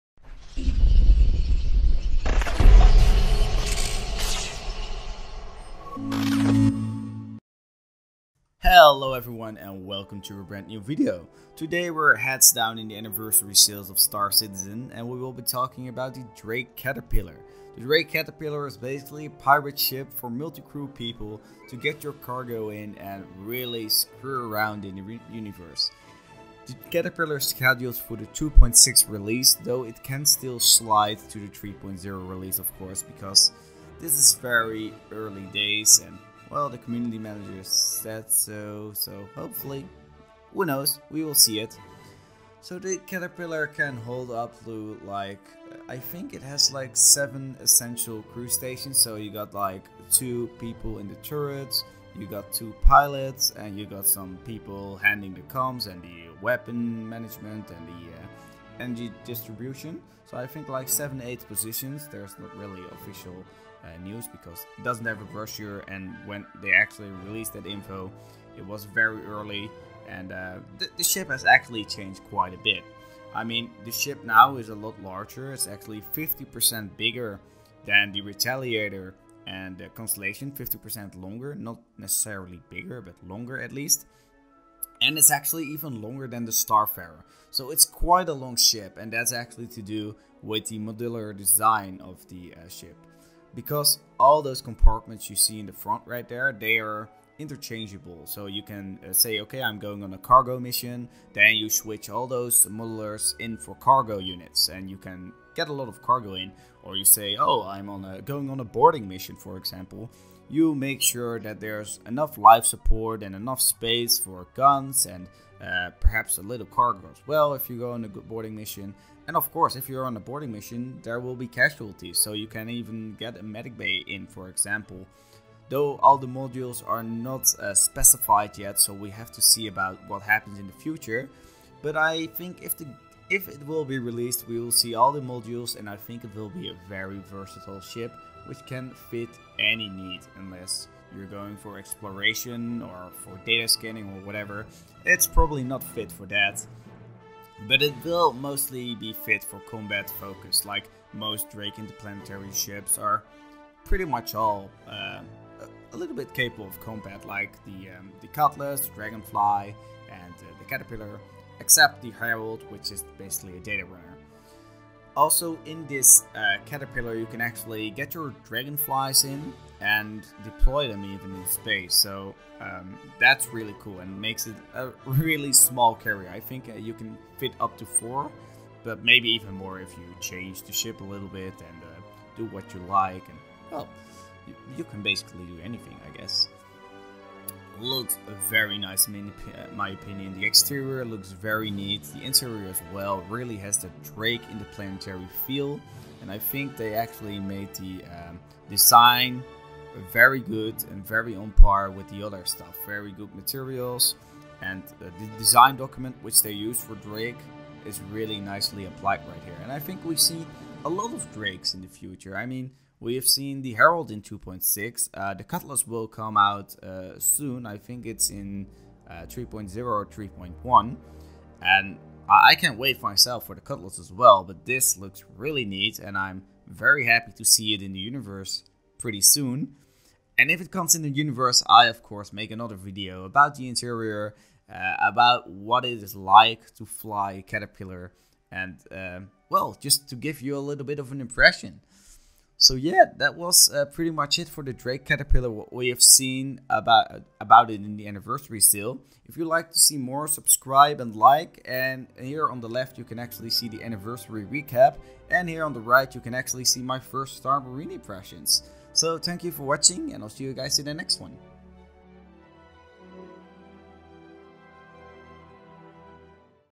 Drake Caterpillar First Impressions - Drake Anniversary Line-up - Cutting Edge Modularity
A first impression on the Drake Caterpillar coming out in update 2.6/3.0. Can't wait to fly/crew this ship! It really looks gorgious!